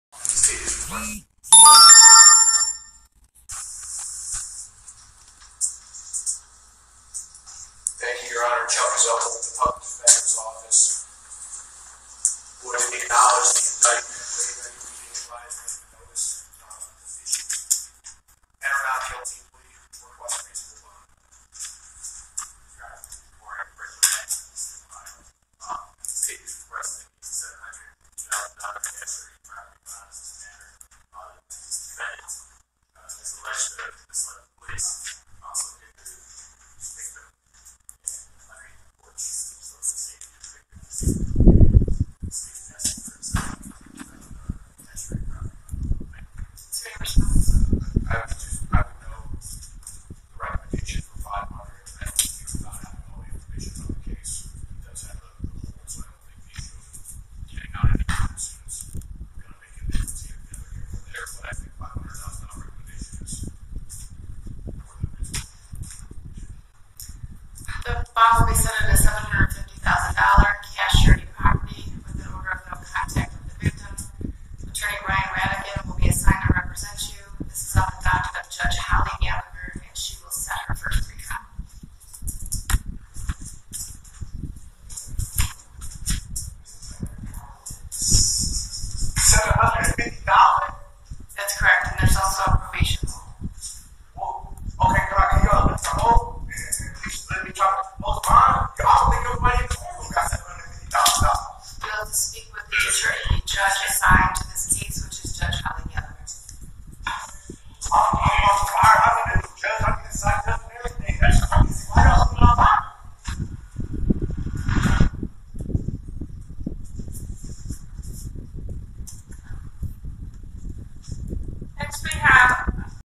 Dalontay Edmond Geiger Sr. took into custody & charged with felonies, including , attempted murder.
Dalontay Edmond Geiger Sr. taken into custody & charged with felonies, including , attempted murder.